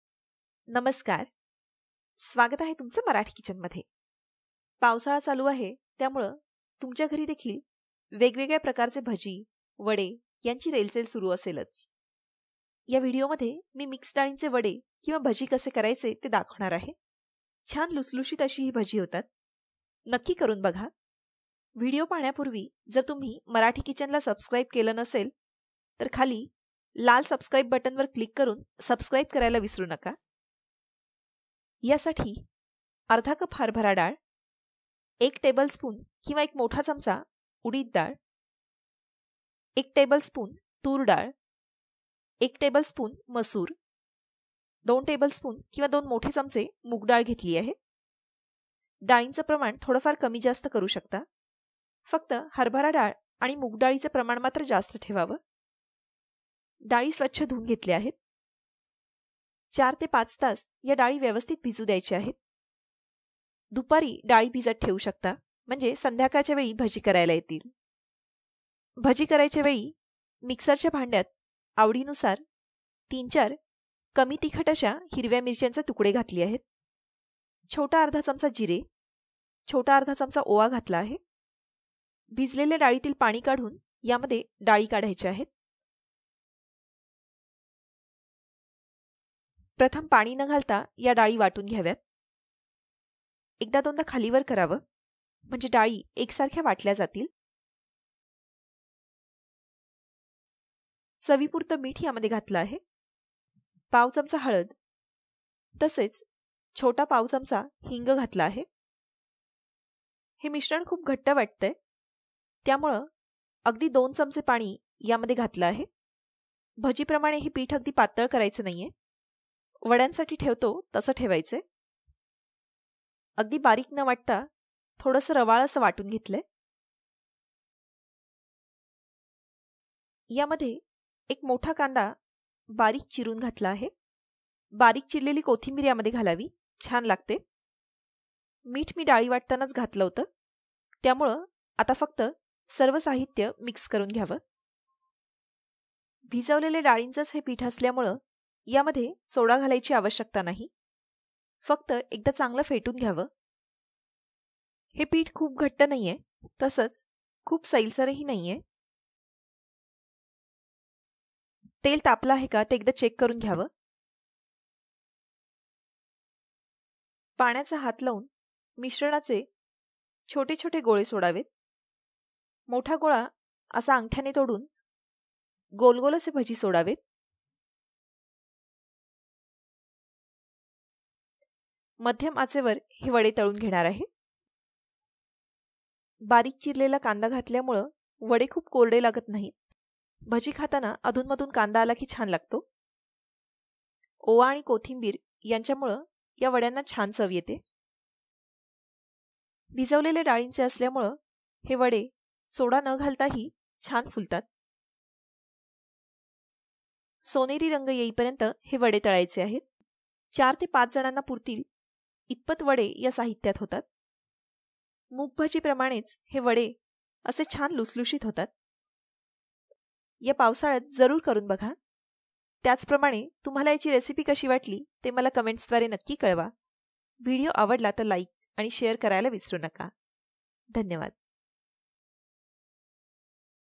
लुसलुशीत मिक्स डाळींचे भजी | मिक्स डाळ वडे | Mix Dal Vade | Mix Dal Bhajji
साहित्य :
१/२ कप हरभरा डाळ
१ tbs तूर डाळ
१ tbs मसूर डाळ
१ tbs उडीद डाळ
२ tbs मूग डाळ
३-४ हिरव्या मिरच्या
१/२ चमचा जिरे
१/२ चमचा ओवा
१/४ चमचा हळद
१/४ हिंग
कांदा
कोथिंबीर